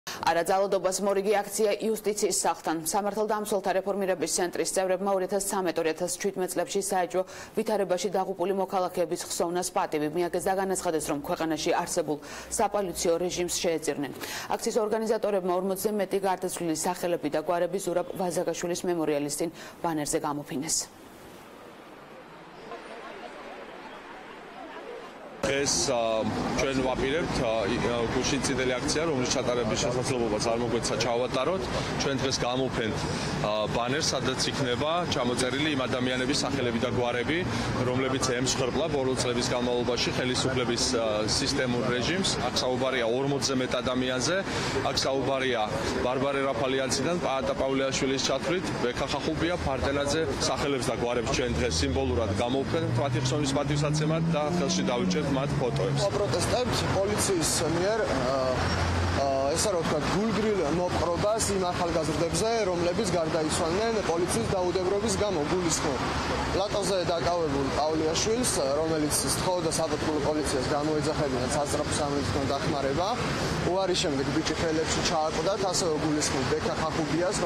"არა ძალადობას" - აქცია იუსტიციის სახლთან
‘’არა ძალადობას’’, მორიგი აქცია იუსტიციის სახლთან. სამართალდამცველთა რეფორმირების ცენტრის წევრებმა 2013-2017 წლებში საეჭვო ვითარებაში დაღუპული მოქალაქეების ხსოვნას პატივი მიაგეს და განაცხადეს, რომ ქვეყანაში არსებულ ”საპოლიციო რეჟიმს” შეეწირნენ. აქციის ორგანიზატორებმა 40ზე მეტი გარდაცვლილის სახელები და გვარები ზურაბ ვაზაგაშვილის მემორიალის წინ ბანერზეე გამოფინეს.